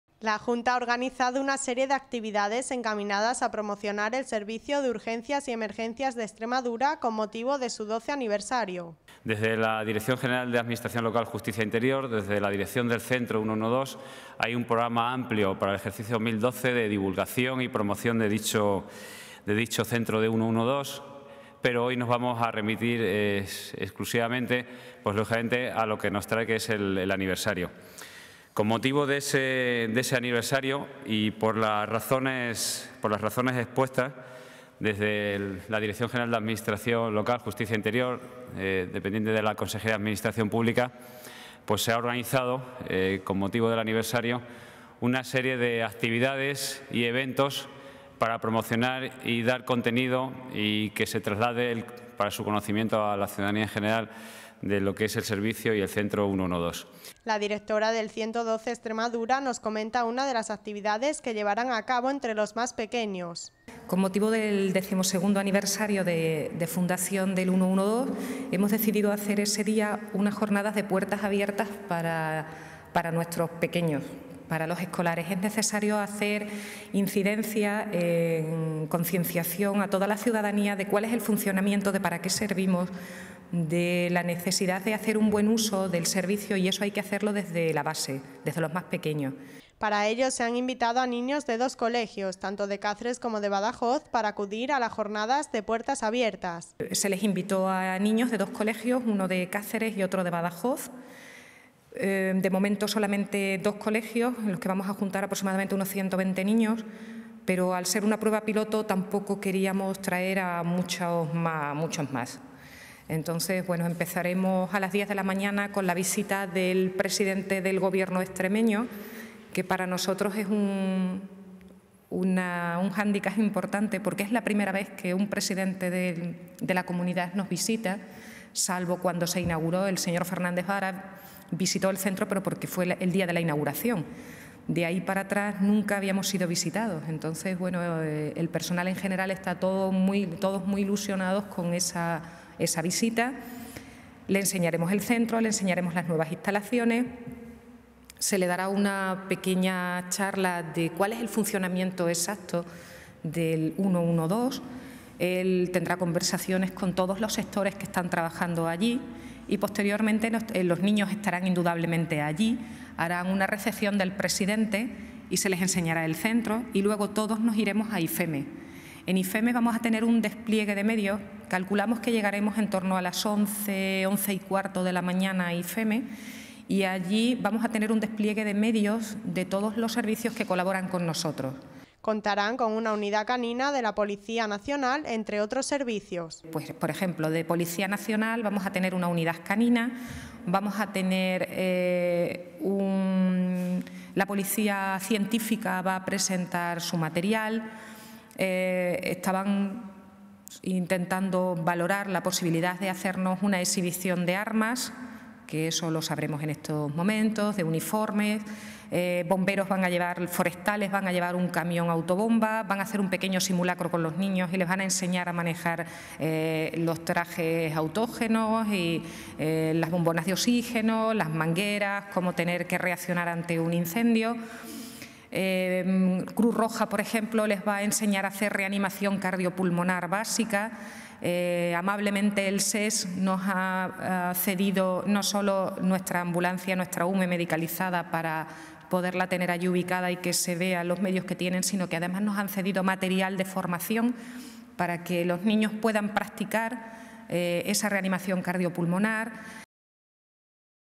Jornada de puertas abiertas en el XII Aniversario del 112 Extremadura
La Junta ha organizado una serie de actividades encaminadas a promocionar el servicio de Urgencias y Emergencias de Extremadura con motivo de sus doce años.

La directora del 112 Extremadura, nos comenta una de las actividades que llevarán a cabo.
Para ello se han invitado a niños de dos colegios, tanto de Cáceres como de Badajoz.
Contaran con una unidad canina de la Policía Nacional, entre otros servicios.
*k30extremadura* *varios* *junta* *bucle*